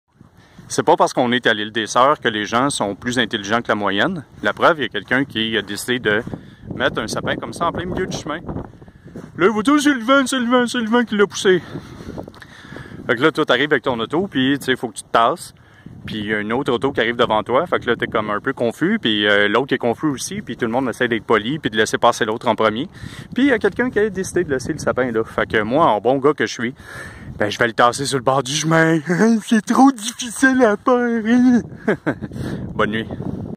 Île des soeurs Temps des Fêtes! Civisme.
Quelqu'un a laissé son sapin de Noël comme ça en plein milieu de la rue! Faire preuve de civisme